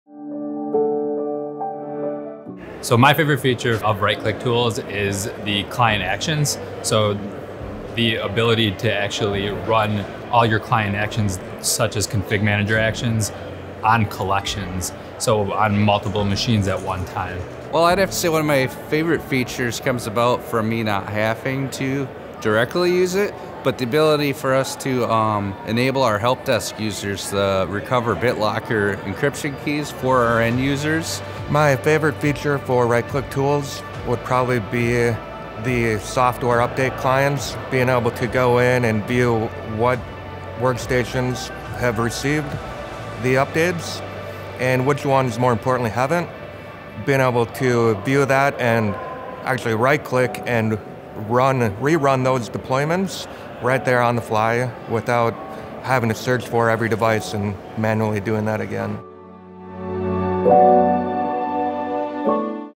Right Click Tools User Testimonials
Hear what some Right Click Tools users have to say about the product.

Have a feature request?